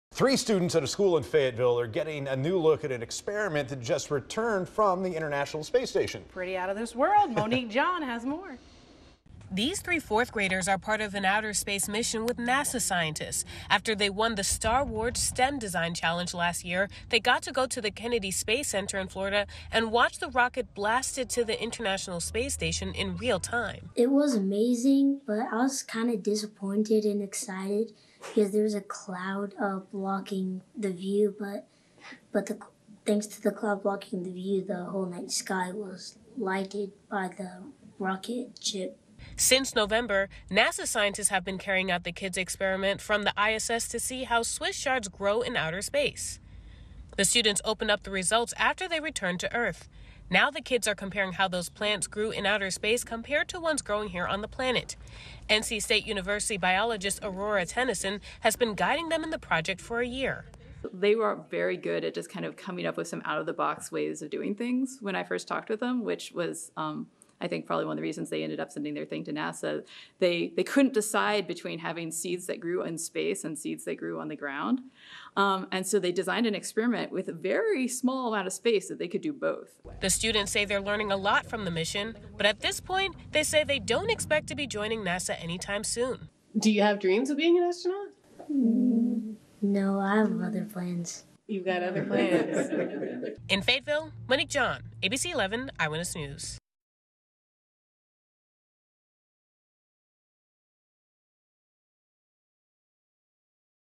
NC students lead science project to and from outer space
Some Cumberland County Schools students are going to infinity and beyond with a science experiment they've been leading with NASA.